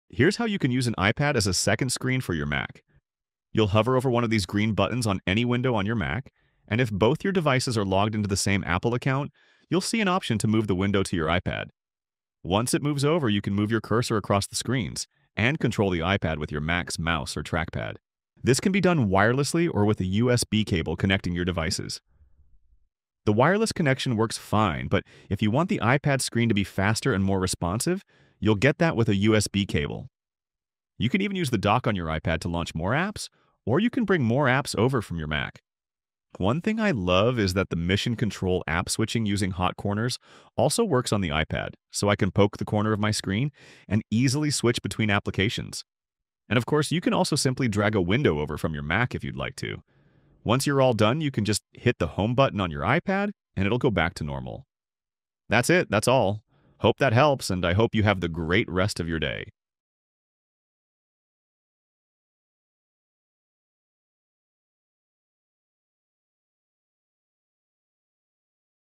How To Use iPad As a Second Monitor – Quick Tutorial
You can use an iPad as a second display for any Mac, but connecting the two might be a little confusing at first. Here's a quick tutorial on how to easily use an iPad as a second screen for your Mac.

Our favorite setup:
• Laptop - MacBook Pro M4
• Desktop - Mac Mini M4
• Monitor - Apple Studio Display
• 2TB SSD Hard drive